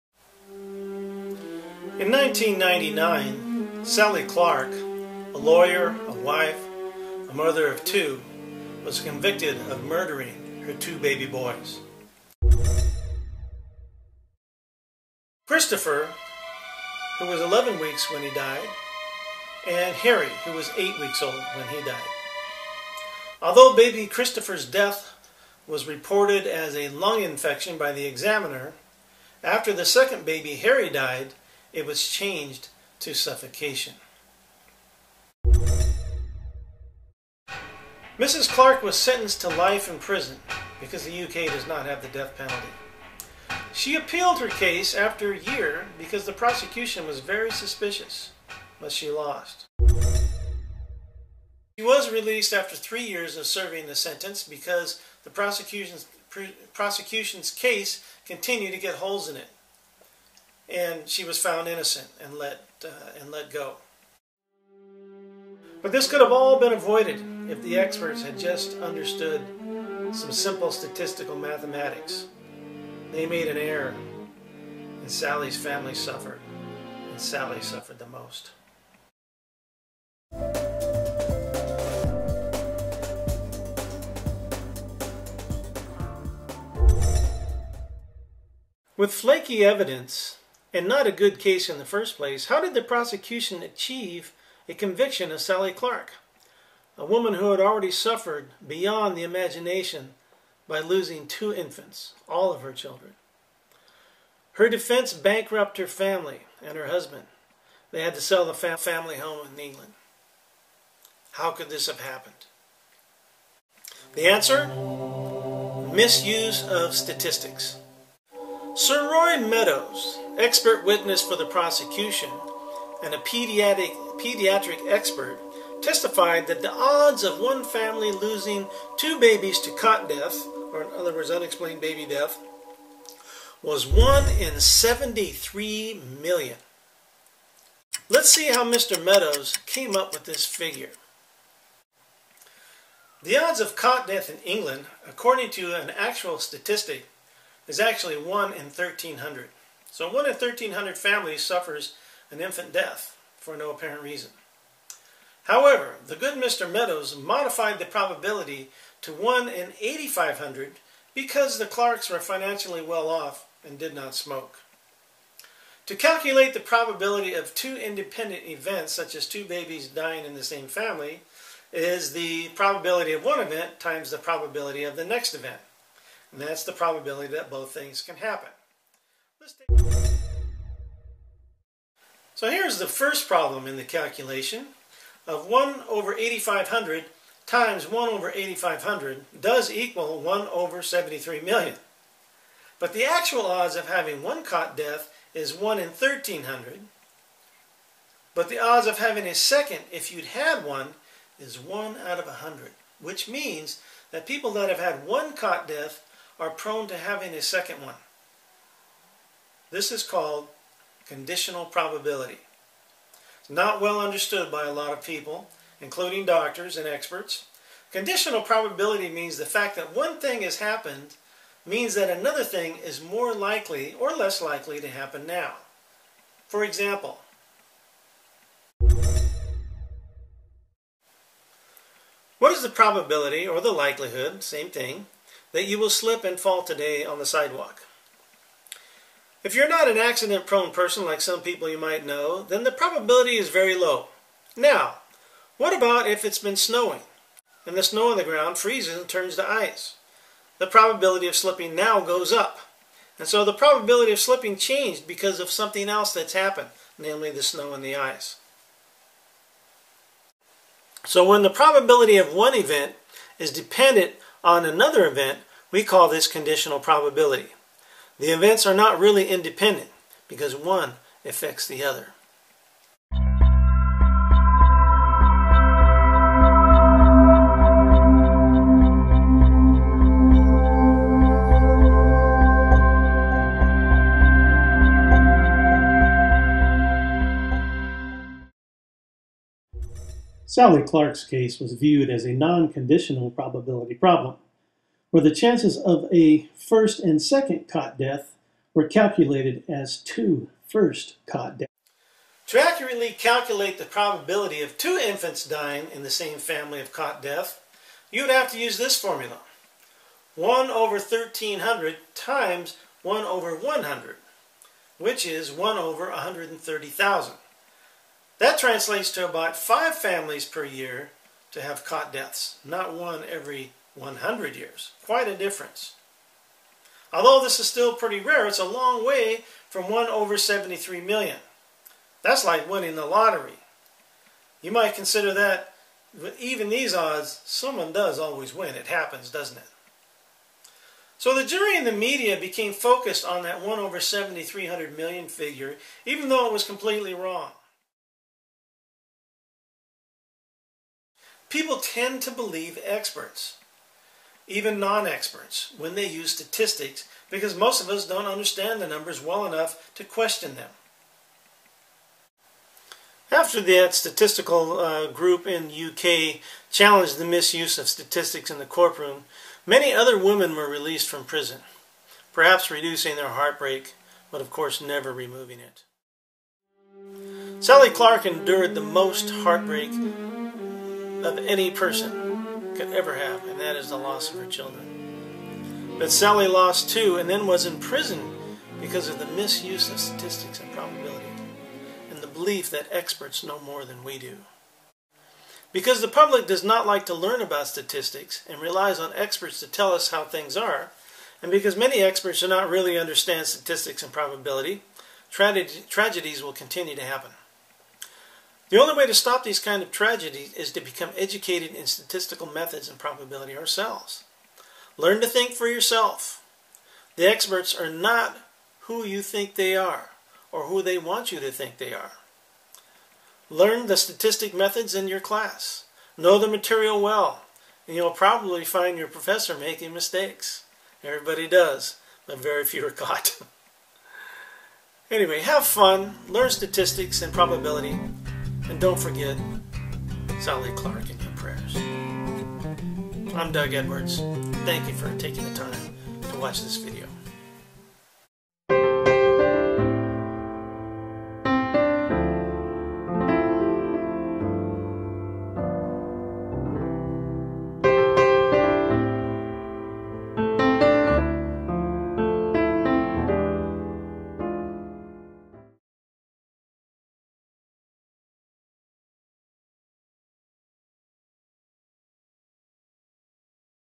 Innocent Mother Sent To Prison over Statistical Error
Sally Clark was convicted of killing her two baby boys after her second baby died at only 8 weeks old.  Although the prosecution was suspicious from the start, they managed to achieve a conviction.  Much of the conviction was based on a probability calculation by Sir. Roy Meadow, a pediatric expert who predicted the likelihood of one family having two infants die of natural causes was one in seventy three million.  Although the judge warned the jury about convicting based on a statistic, that 73 million number was already in the jury's mind.  To make matters worse, Sir Meadows claimed "one cot death tragic, two suspicious and three murder."  His lack of math skills (and the jury's as well), plus his dramatic statements served to send an heart-broken and innocent women to prison for the rest of her life.  In prison, she was the target for other prisoners because of her reputation as a baby killer, the daughter of a policeman and an attorney.  Although Sally Clark's first appeal was lost after serving about one year in prison, she was eventually released after it was found that the prosecution was full of holes.  A small victory for Sally, but not enough, for shortly after she was released from prison, she was found dead of alcohol poisoning.  She was never able to overcome the grief of loosing her two only children, being convicted wrongly of their murder, enduring the ridicule and harassment in prison and then released from prison as if no crime had been committed against her.